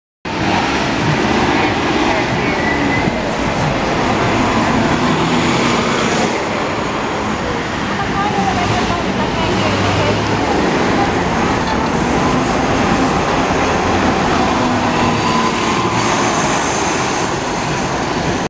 (MTA NYC Bus) Riverbank Park bound 2012 Novabus LFSA's #5280 & #5985 on the Bx19
Failed attempt on 5280, but oh well. ¯\_(ツ)_/¯

5985 a recent transfer from KB (and for some reason, signed as Subway Shuttle).